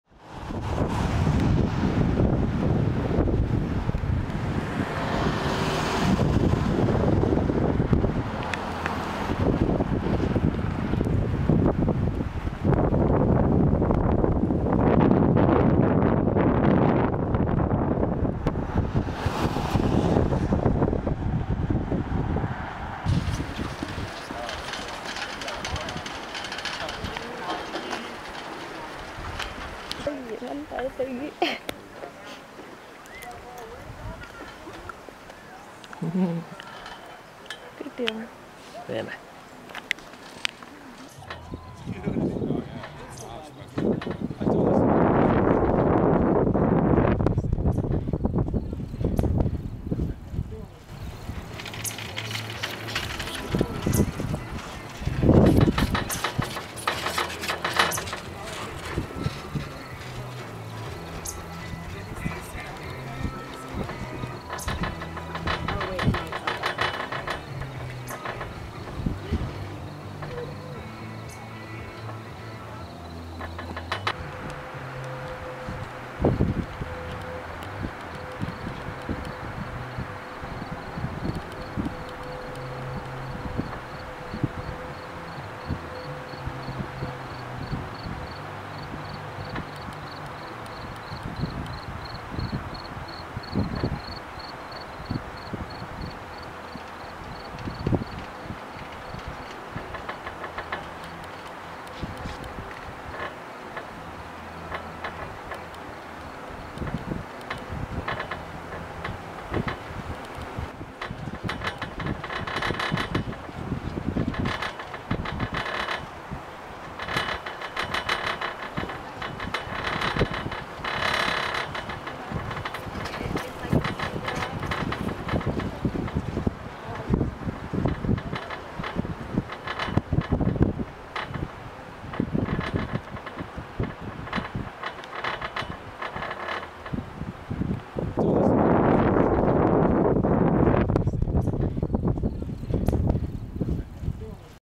Scary Scenic Ski Lift Ride - Mount Baldy Chair Lift California
Experience the beauty of a scenic ski lift ride at Mount Baldy in California. This video captures the stunning views and peaceful atmosphere of a chair lift ride through the mountains. Enjoy the serene sights and sounds of nature as you take a virtual journey to the top of the slopes.
More videos @journeylogsvideos